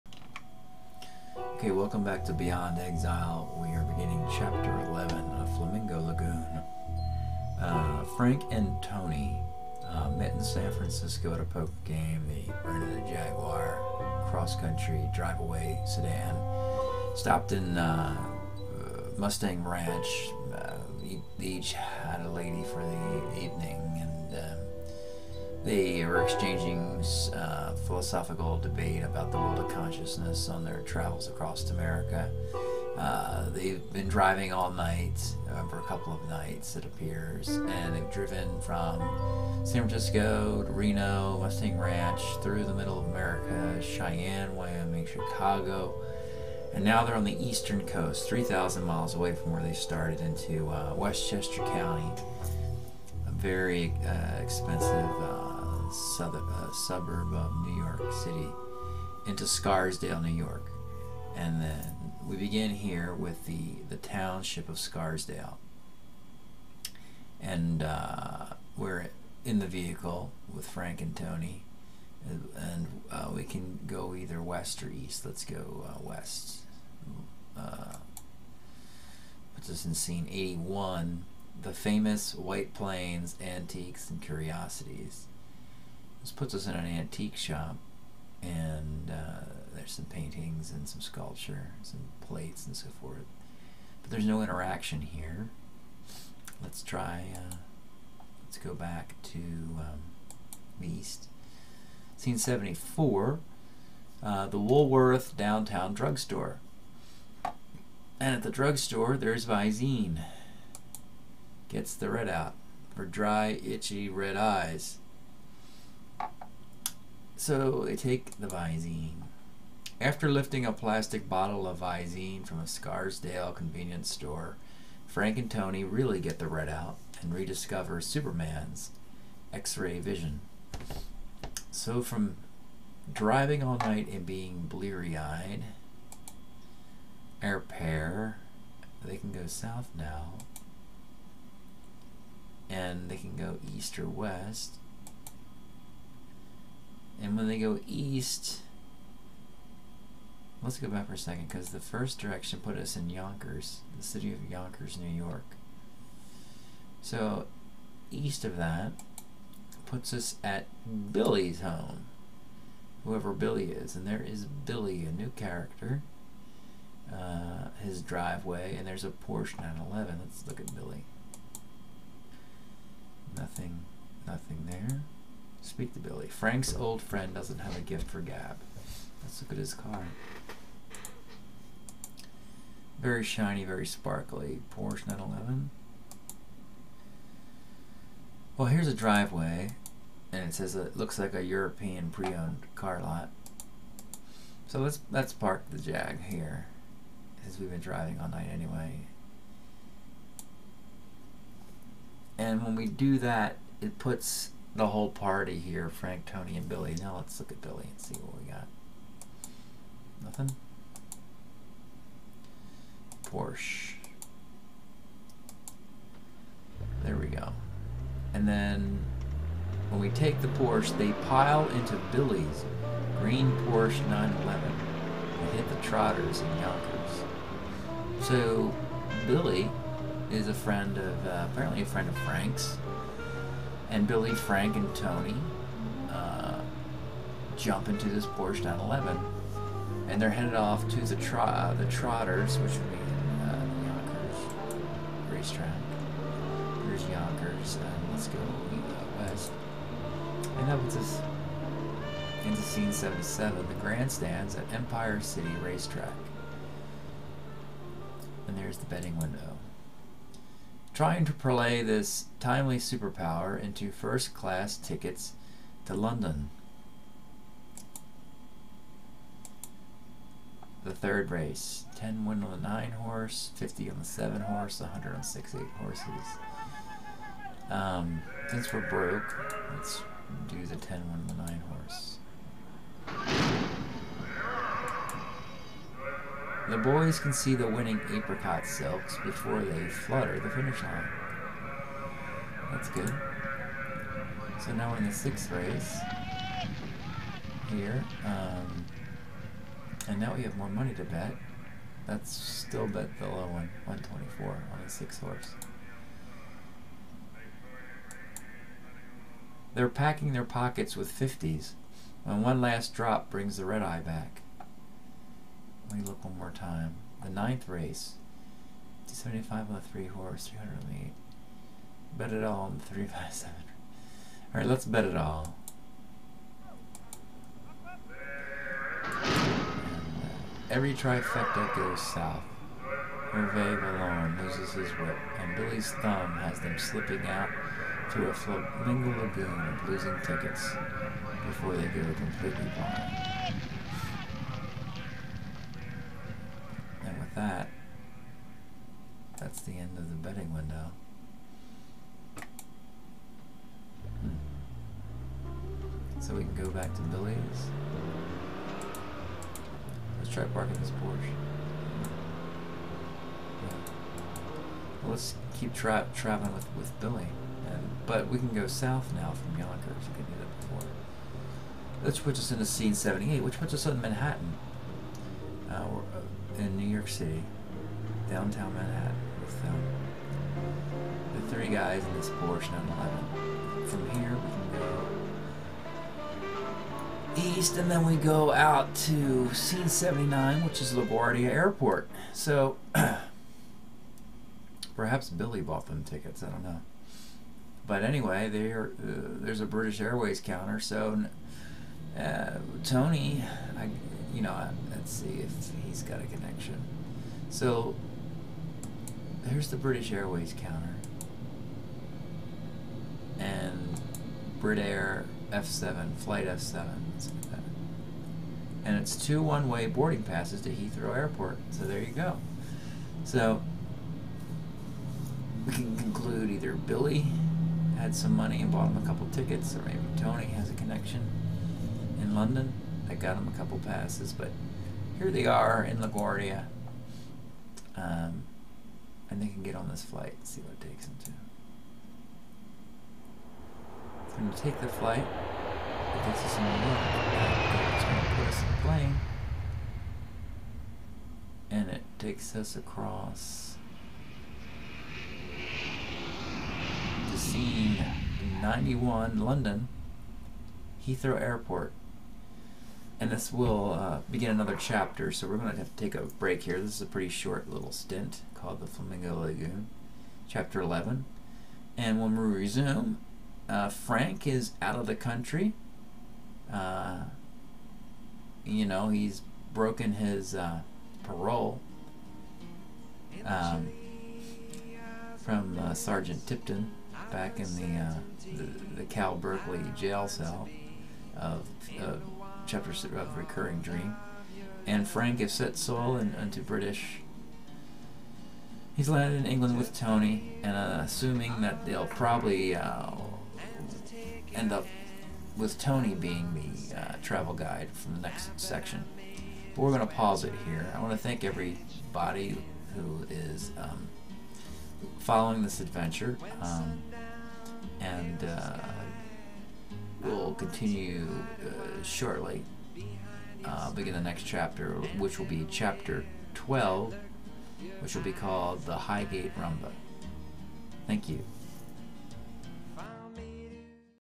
BEYOND EXILE Chapter 11 - "A Flamingo Lagoon"
SCARSDALE, NEW YORK - After driving cross country, Frank and Tony visit "Billy Collins" and head for the racetrack in Yonkers.(1971~1974) Featuring music by Stan Getz and Donovan.